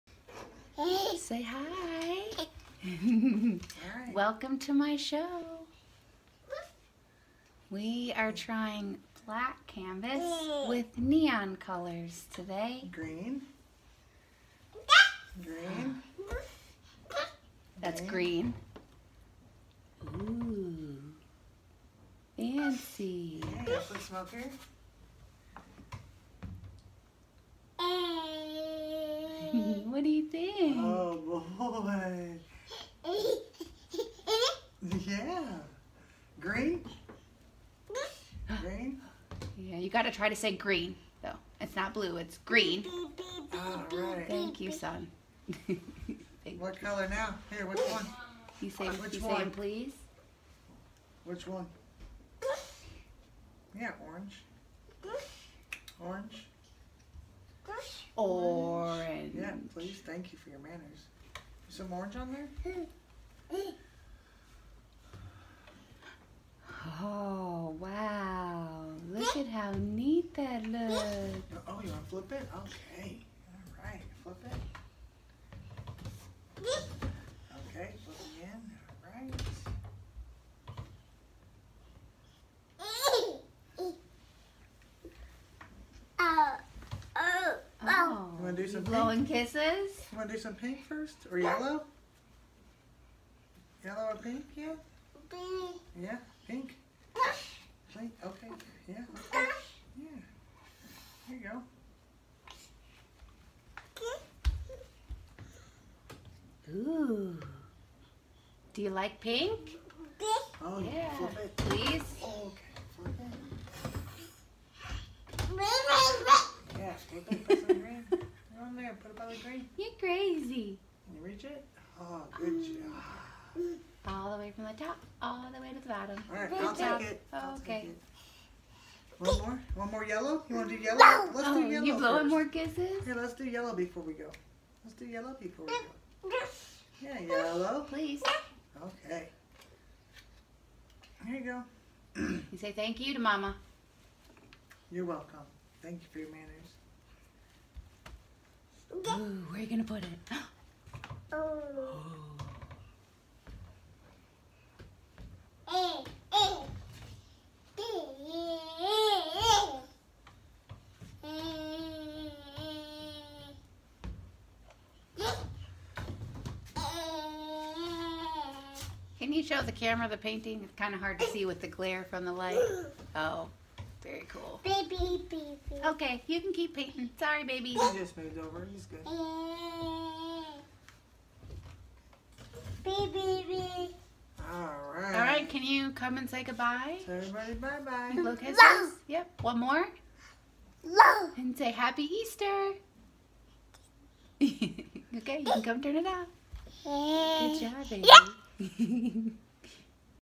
Day 28: HAPPY EASTER!!! just started a painting on a black canvas!
We decided to have Kahlo paint fluorescent colors on a black canvas for Easter! 🎨🤙🏽